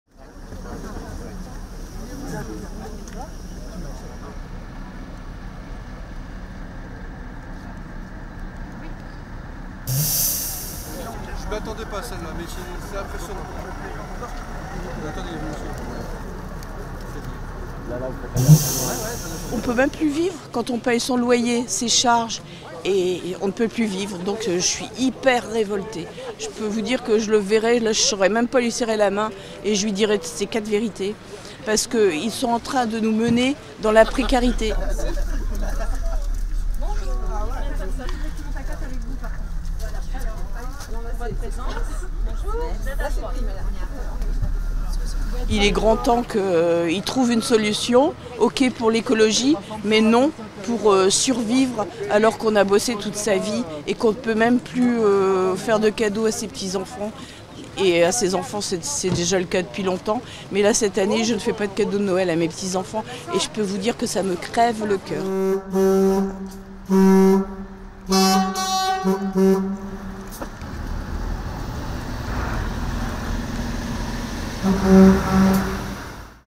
Bus carrying 'yellow vest' protesters depart for Paris
A bus carrying protesters leaves the Lille area in northern France to take part in a protest in Paris. "Yellow vests" protesters, dressed in high-visibility jackets, are set to cause another day of disruption on Saturday amid calls to bring Paris to a standstill.